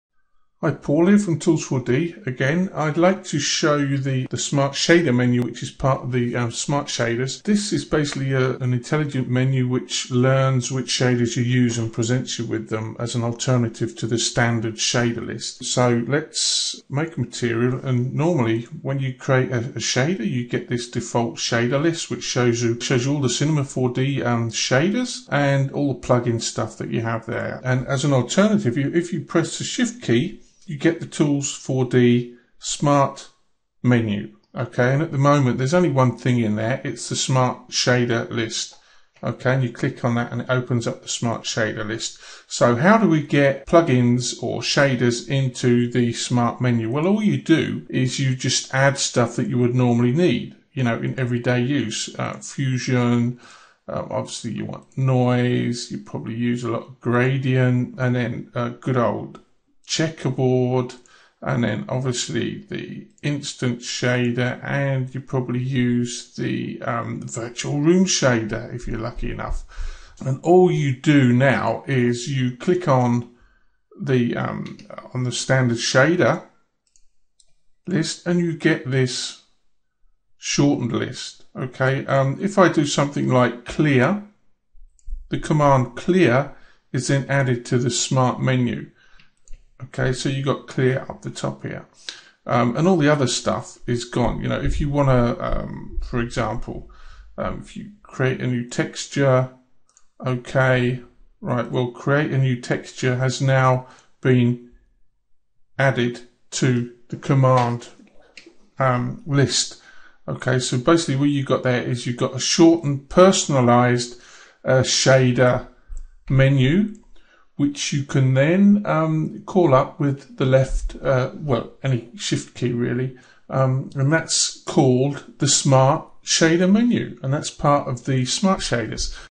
Tools4D smart shader menu.
An intelligent shader menu for C4D. It learns what shaders you use , on the fly presents you with a personalized popup shader menu.